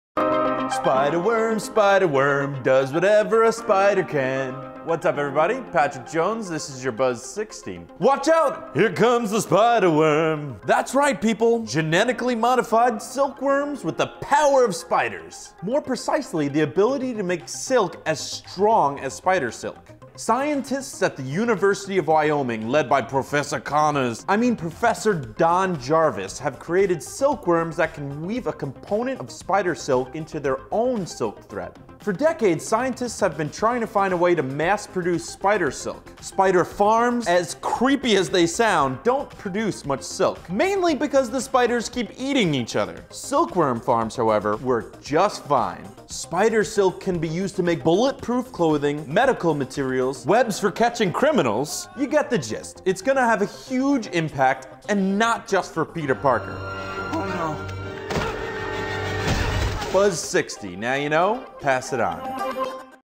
Spider Silk Ready For Mass Production
Scientists from The University of Wyoming under Professor Don Jarvis have successfully genetically modified silk worms to combine components of spider silk with their own silk to create a composite material with mass appeal.  This could be the solution to the demand for spider silk, which has countless uses beyond just helping Spider-Man. Patrick Jones explains why silk worms are the answer.

Do you like our videos? Then you'll love more Buzz60.